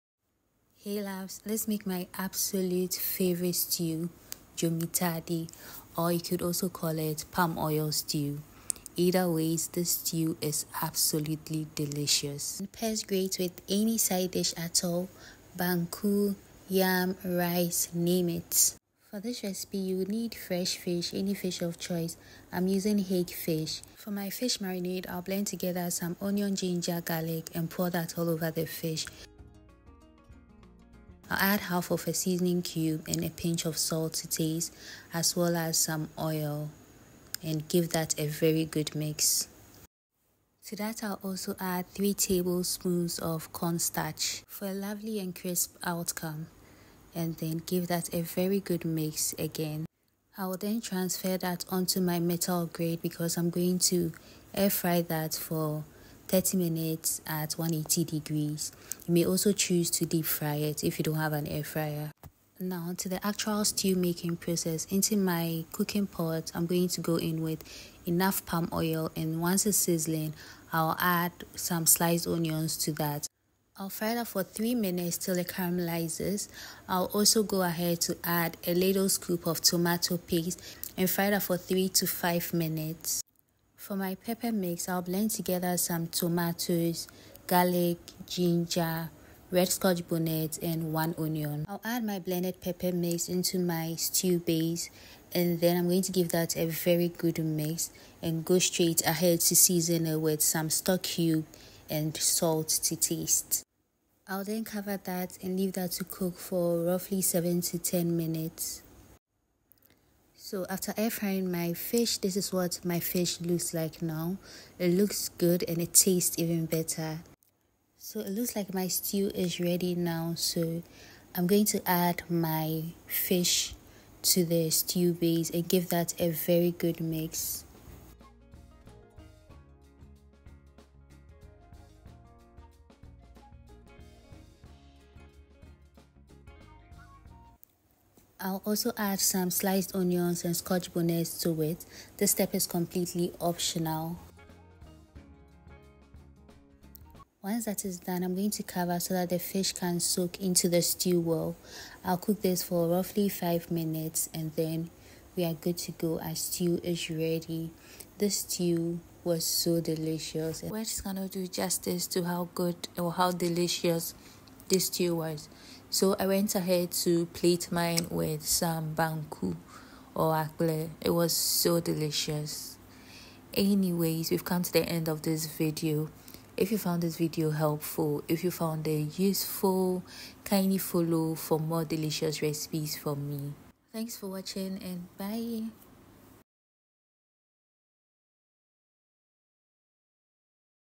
PALM OIL FISH STEW (DZOMI TADI)
Let’s make this simple but absolutely delicious Palm Oil Stew (Dzomi Tadi / Atadi) recipe. Pairs great with any side dish as well 😍